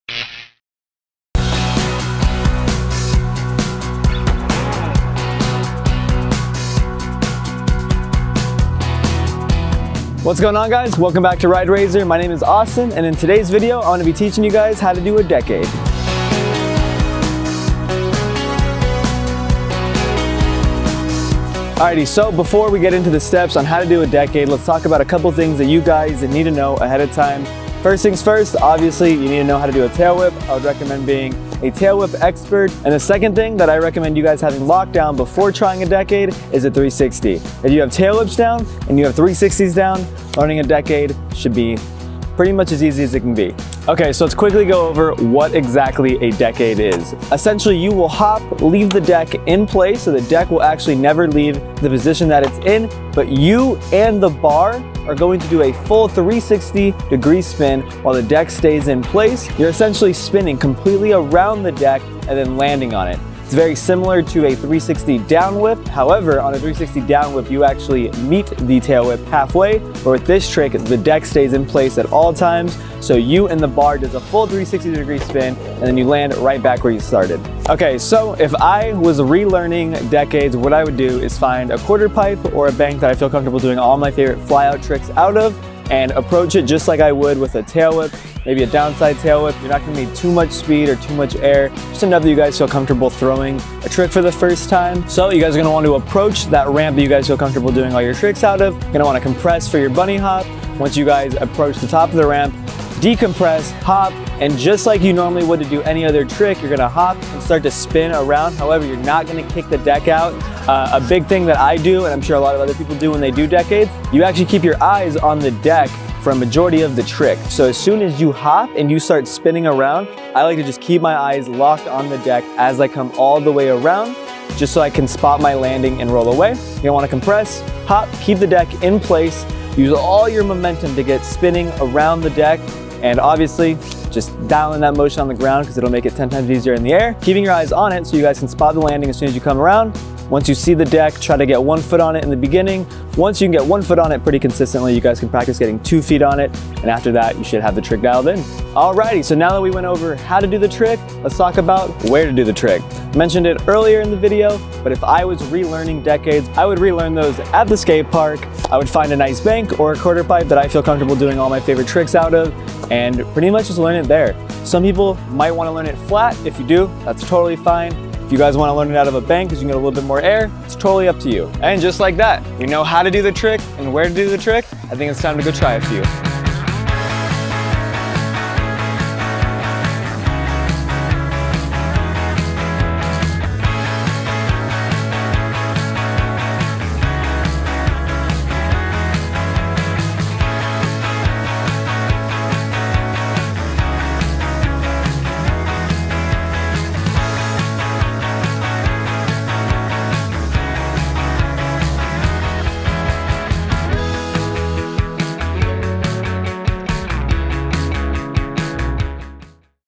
How To Decade - RideRazor
Learn how to Decade from our Razor pro Austin Spencer.